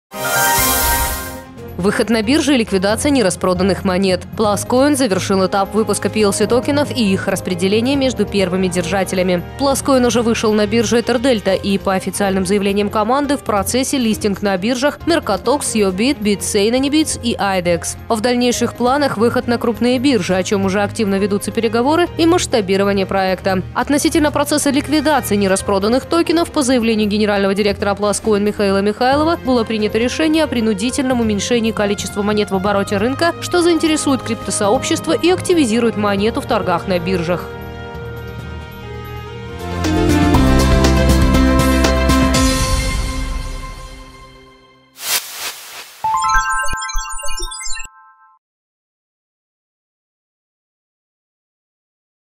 Выход на биржи и ликвидация нераспроданных в ICO токенов PlusCoin
Выход на биржи и ликвидация нераспроданных монет - PlusCoin завершил этап выпуска PLC токенов и их распределения между первыми держателями. PlusCoin уже вышел на биржу Etherdelta и, по официальным заявлениями команды, в процессе листинг на биржах Mercatox, Yobit, Bitsane/Anybits и IDEX. В дальнейших планах выход на крупные биржи (о чем уже активно ведутся переговоры) и масштабирование проекта. Относительно процесса ликвидации нераспроданных токенов, по заявлению генерального директора PlusCoin Михаила Михайлова, было принято решение о принудительном уменьшении количества монет в обороте рынка, что заинтересует криптосообщество и активизирует монету в торгах на биржах.